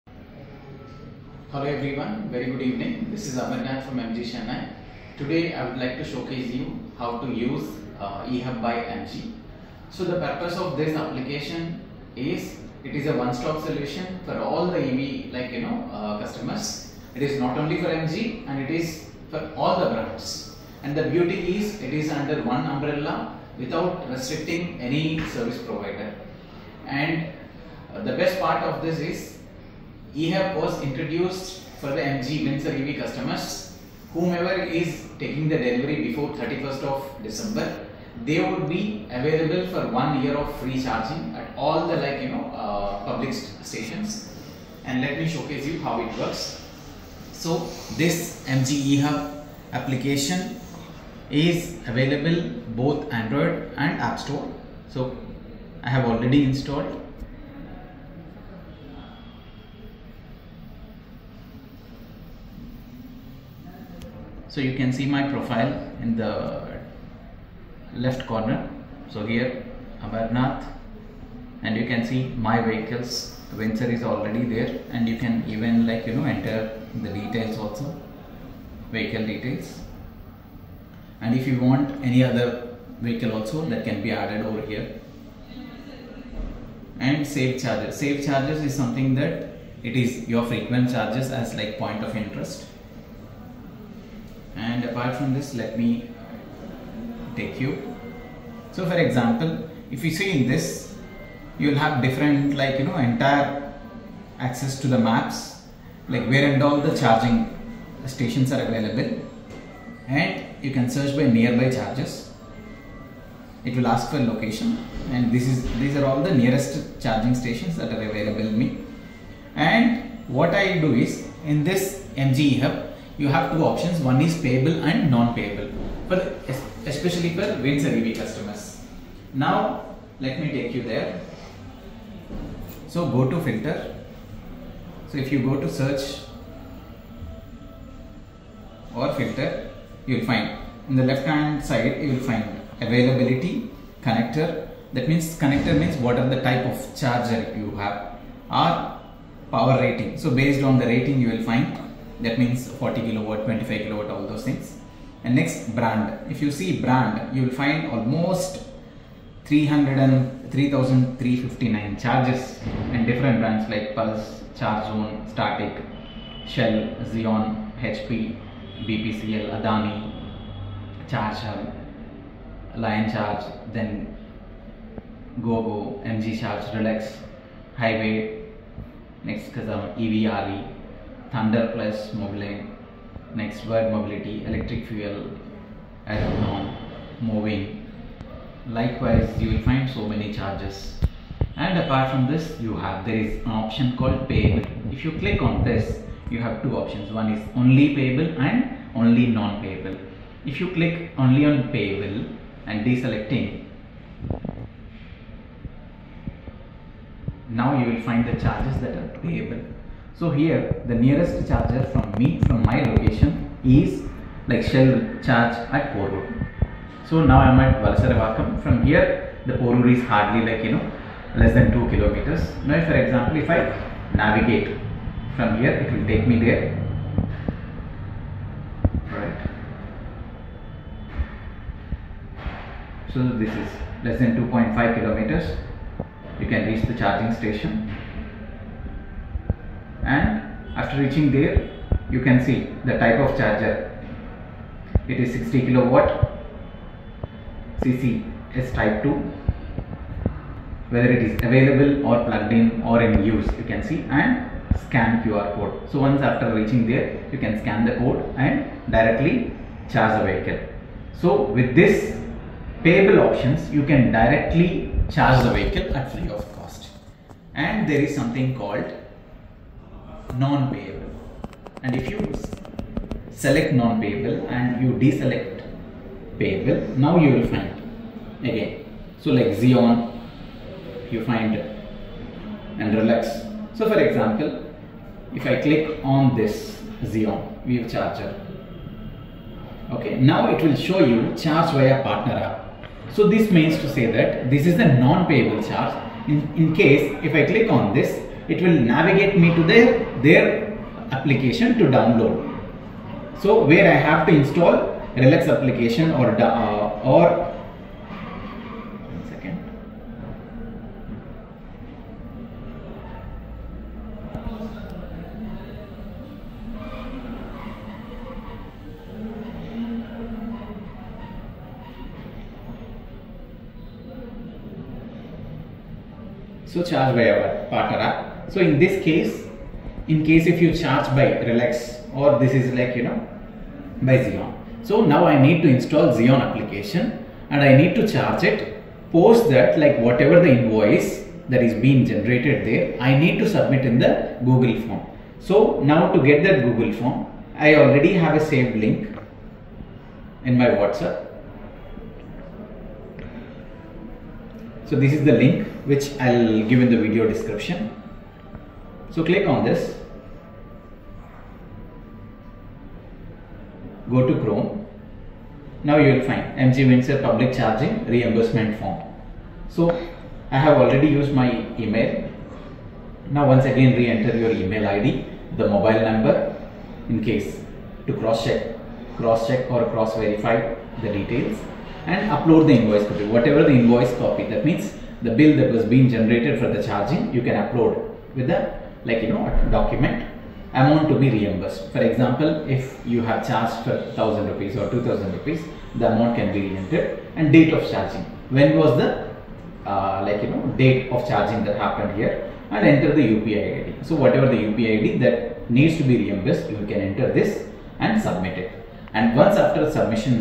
EHUB - MG MG EV windsor ZS EV evs tata mahindra hyundai byd All evs
eHUB by MG is designed to be a universal platform, integrating various charging networks under one umbrella. This integration allows EV owners to discover, access, and utilize a vast network of charging stations without being restricted to a single provider. By encompassing 80% of the EV fast chargers in India, eHUB by MG significantly reduces range anxiety, a common concern among EV users. This extensive coverage ensures that EV drivers can find charging points easily, whether they are in urban centers or traveling across the country. So for the free charging for your Windsor vehicle, you can go into MG application and click on the filter option in the left hand side corner and then click on into Payable chargers and whichever options are available that will be free for you.
And for the non-payble chargers such as JIO-BP, Statiq and others as of now you need to pay the amount at the chargers but it will get refund to you within 2-3 business days and to do get that refund you need to fill this google form